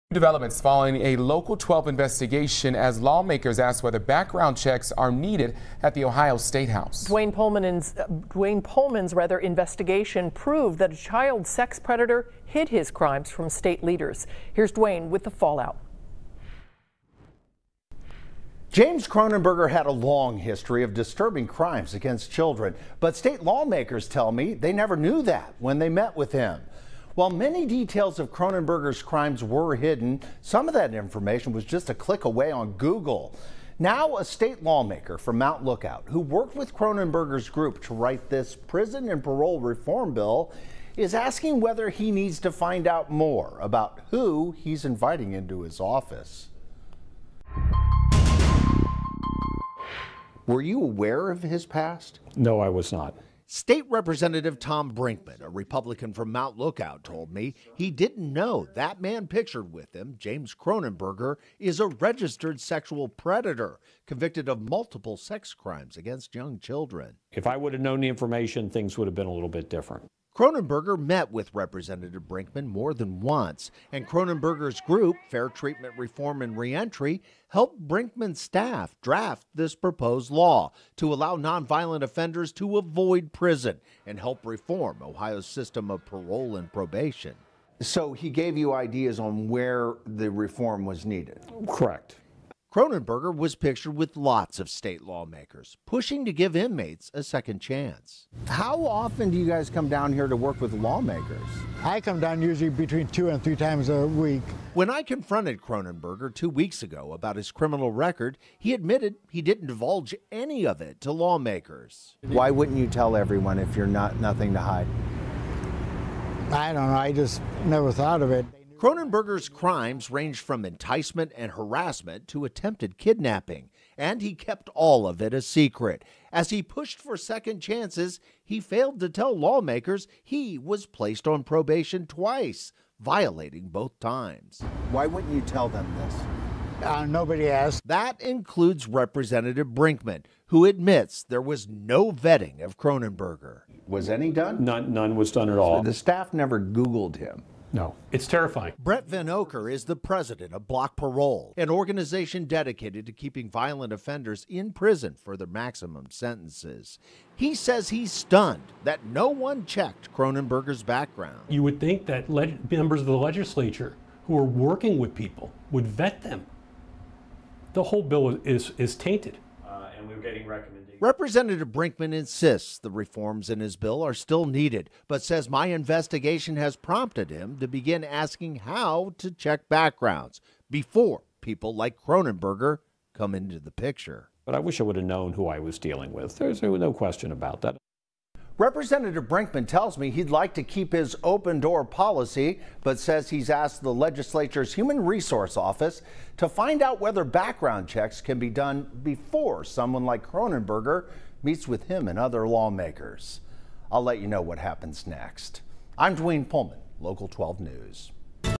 Local 12 investigation leads lawmakers to consider background checks at Ohio statehouse
COLUMBUS, Ohio (WKRC) - A day after a Local 12 Investigation revealed that a child sex predator hid his criminal background when he met with some of Ohio’s most powerful lawmakers and other leaders, State Rep. Tom Brinkman says he has asked whether better background checks are needed to find out who is really meeting with legislators.

NOT AWARE

Brinkman made it clear he was not aware James Kronenberger, the founder of Fair Treatment Reform and Re-entry (FTRR), was a registered sexual predator who had been found guilty several times over nearly four decades for sex crimes against young children.

CLICK HERE FOR LOCAL 12 INVESTIGATION, “HIDDEN PAST”
“If I would have known the information, things would have been a little bit different,”Brinkman said, conceding he not only met with Kronenberger, but worked with FTRR to draft a prison and parole reform bill -- a proposed mandate reforming Ohio’s prison system as well as providing early parole to non-violent offenders.

READ BRINKMAN’S DRAFT BILL FOR PRISON AND PAROLE REFORM
When I spoke with Kronenberger nearly two weeks ago, he said he often met with lawmakers, including Brinkman, as he pushed for prison and parole reform at Ohio’s statehouse. When asked how often he met with lawmakers, Kronenberger told us, “I come down here, usually two to three times a week.”

NOBODY ASKED

Kronenberger admitted he didn’t tell any of the state’s powerful lawmakers about his convictions for sex crimes against children as young as 5 years old, nor did he discuss details of his convictions for enticement, attempted kidnapping and harassment that led to him spending nearly 25 years in prison.

When asked why he withheld the information, he said, “I just never thought of it,” adding, “Nobody asked.”

Brinkman confirmed he and his office never vetted Kronenberger. No one on his staff even conducted a simple Google search.

“None was done at all,” Brinkman conceded.

TERRIFYING

“It’s terrifying,” said Bret Vinocur, the founder and president of Block Parole, Inc, an organization dedicated to keeping violent offenders in prison for their maximum sentences.

“You would think that members of the legislature who are working with people would vet them,” Vinocur said, after learning that lawmakers never conducted a basic search to find out who Kronenberger was.

Vinocur then quickly turned to question the validity of Brinkman’s bill calling for prison reform.

“The whole bill is tainted,” Vinocur said.

BACKGROUND CHECKS?

But Brinkman insists prison and parole reforms outlined in his bill are still needed but says the fact that Kronenberger didn’t disclose his disturbing past is a big problem.

“I wish I would have known who I was dealing with. There’s no question about that,” he said.

Now, Brinkman confirms he has asked the human resources office at the Ohio legislature to determine whether background checks should be made available to lawmakers before they meet with individuals like Kronenberger.